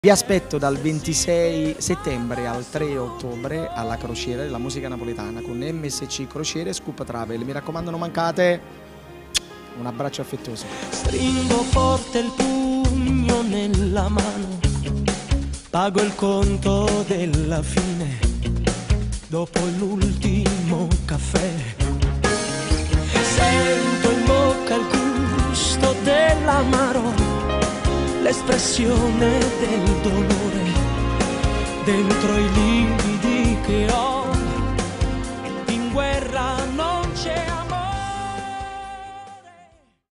Crociera della musica napoletana - promo Sal Da Vinci
Sal da Vinci ci saluta così, dandoci appuntamento il 26 settembre per la partenza della 9°a Crociera della Musica Napoletana!

Info e prenotazioni:
SCOOP TRAVEL SERVICE
Via Gianlorenzo Bernini 90a
80129 Napoli